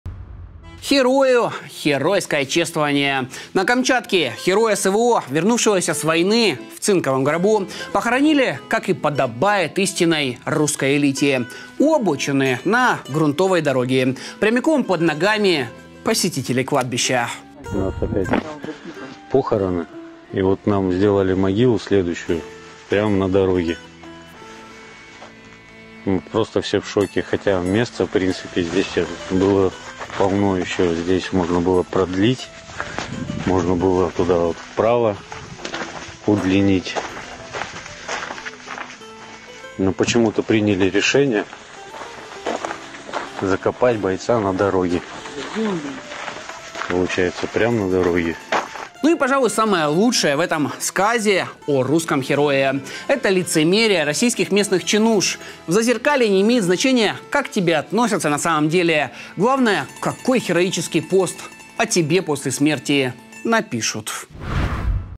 Судьба российского «хероя»! Одно из ХУДШИХ ЗАХОРЕНЕНИЙ "ГЕРОЯ СВО" | В ТРЕНДЕ
«Херою» - херойское чествование! На Камчатке – «хероя СВО» вернувшегося с войны в цинковом гробу… похоронили, как и подобает истинной русской элите… у обочины, на грунтовой дороге… прямиком под ногами – посетителей кладбища!